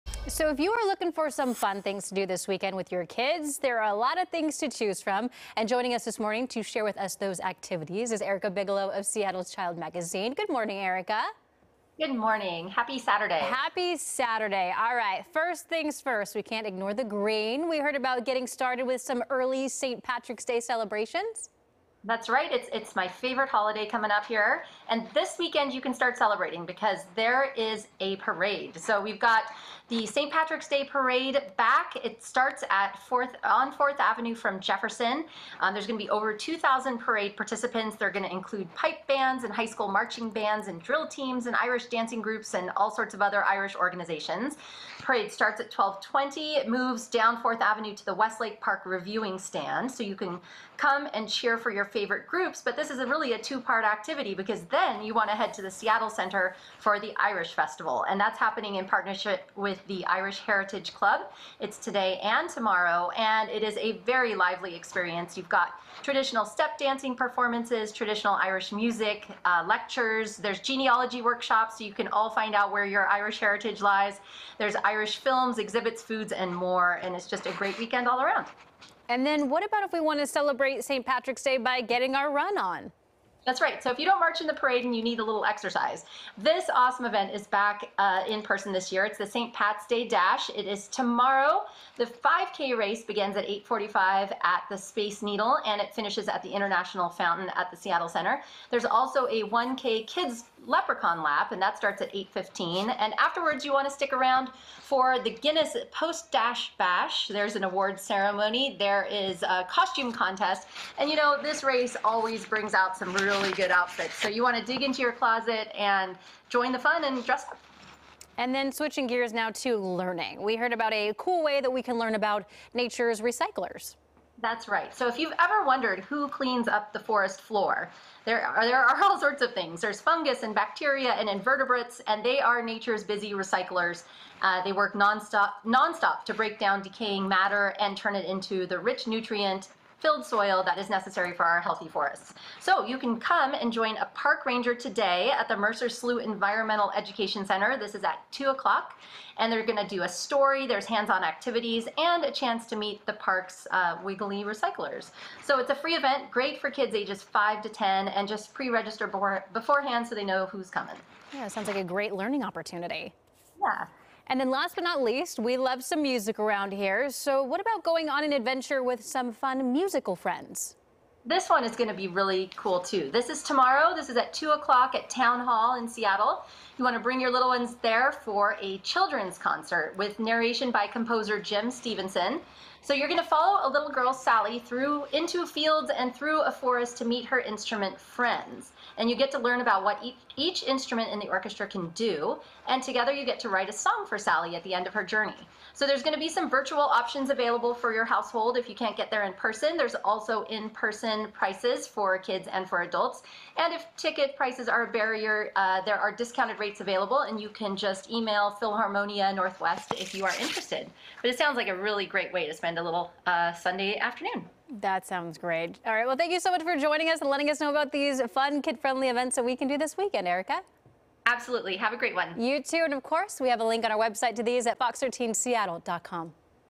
Seattle's Child: St. Patrick's Day fun, outdoor activities | FOX 13 Seattle
If you're looking for some fun things to do with your kiddos this weekend, you're in luck—because St. Patrick's Day is this week! FOX 13's Chynna Greene is joined by Erika Bigelow of Seattle's Child Magazine, with things to get you outdoors with the family.